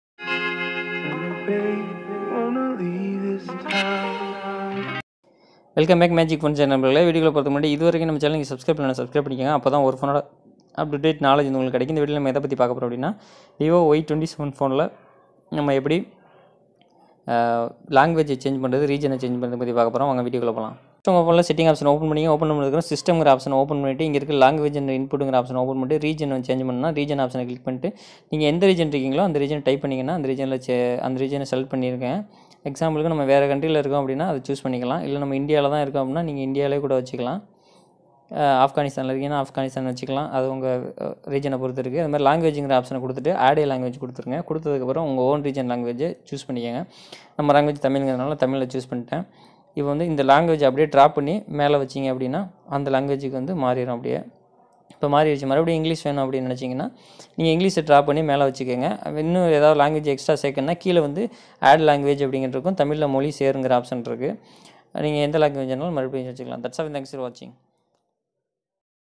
How to change language and region in VIVO Y27 in Tamil
In today's video i am going to show How to change language and region in VIVO Y27 phone in Tamil full guide watch full video for acqurecy. unless you have to face a problem..